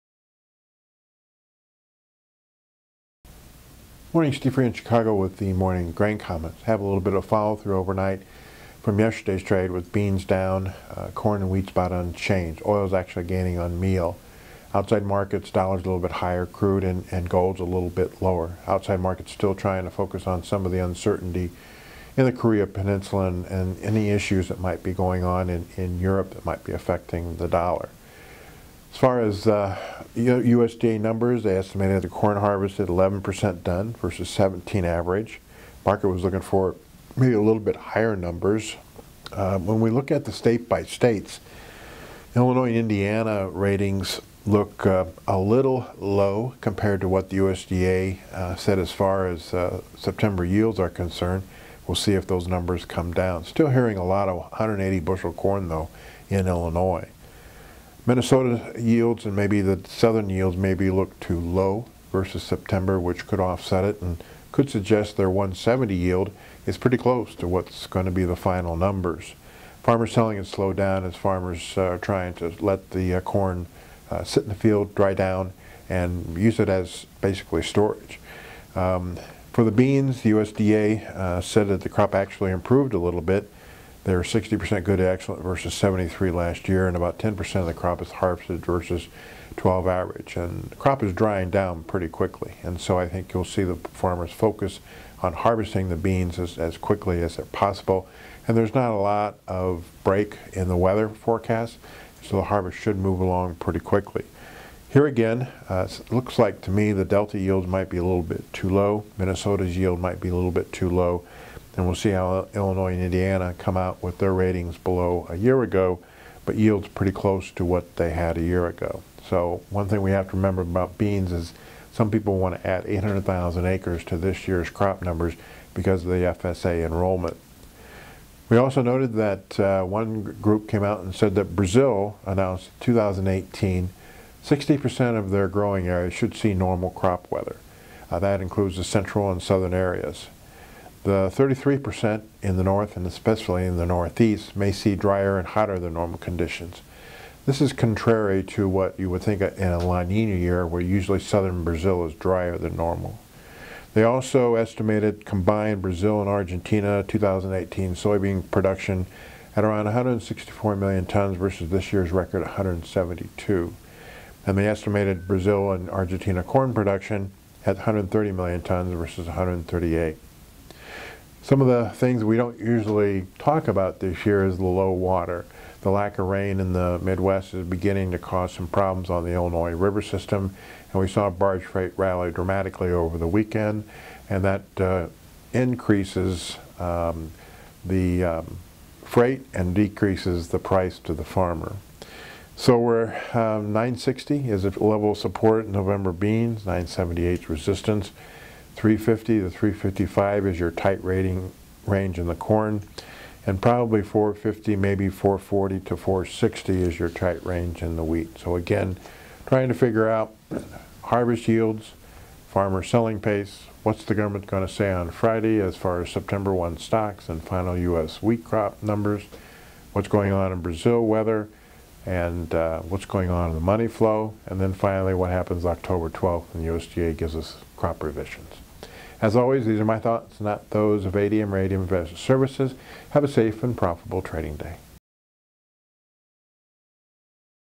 Sept 26 ADMIS Today TV feat Steve Freed on Ag Market Outlook
Steve Freed, Vice President of Grain Research for ADM Investor Services, shares his unique perspectives on the agricultural futures markets from his offices in the historic Chicago Board of Trade.